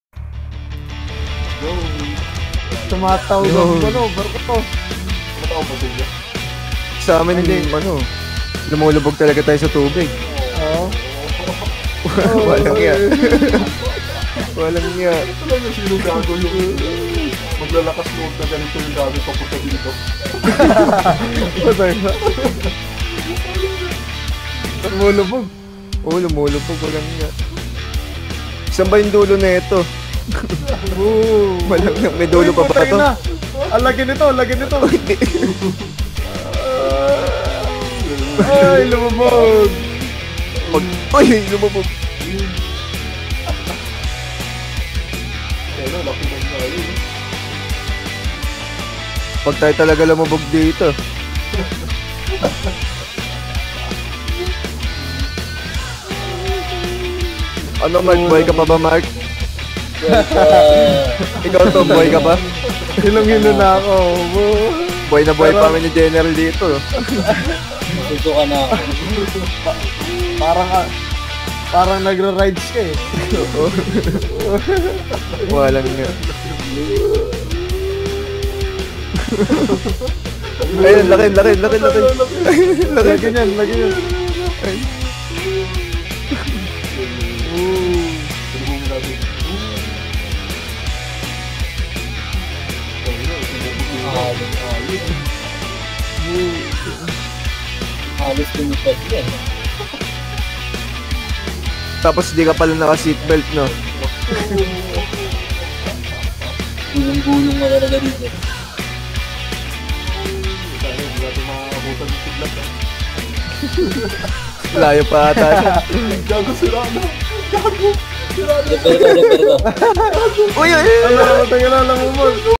Nakipaglaro sa Alon
Sinugod ang bagyo at matataas na alon
Kulitan Sa Custer Province